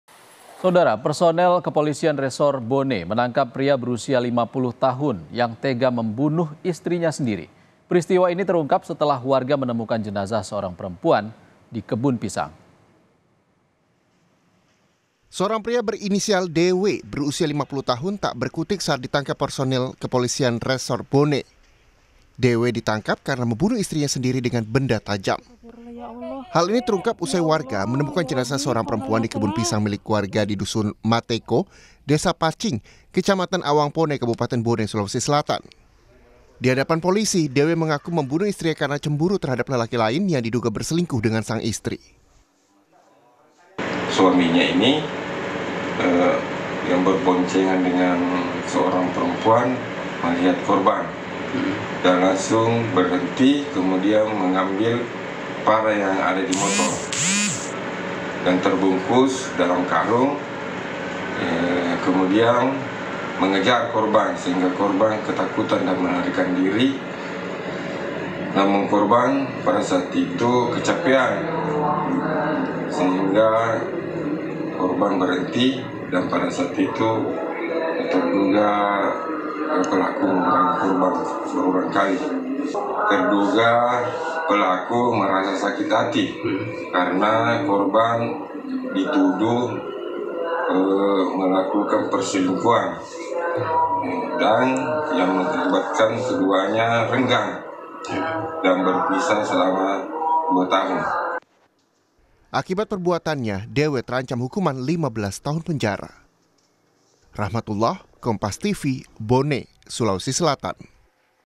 Suami Tega Bunuh Istri Karena Cemburu Buta, Jenazah Istri Ditemukan di Kebun Pisang
BONE, KOMPAS.TV - Seorang pria berinisial DW berusia 50 tahun tak berkutik saat ditangkap personel Kepolisian Resor Bone.

DW ditangkap karena membunuh istrinya sendiri dengan benda tajam.

Hal ini terungkap usai warga menemukan jenazah seorang perempuan di kebun pisang milik warga di Dusun Matekko, Desa Paccing, Kecamatan Awangpone, Kabupaten Bone, Sulawesi Selatan.

Baca Juga Dituding Mabuk dan Aniaya Suami, Istri di Palembang Lapor Balik di

Di hadapan polisi, DW mengaku membunuh istrinya karena cemburu terhadap lelaki lain yang diduga berselingkuh dengan sang istri.

Akibat perbuatannya, DW terancam hukuman 15 tahun penjara.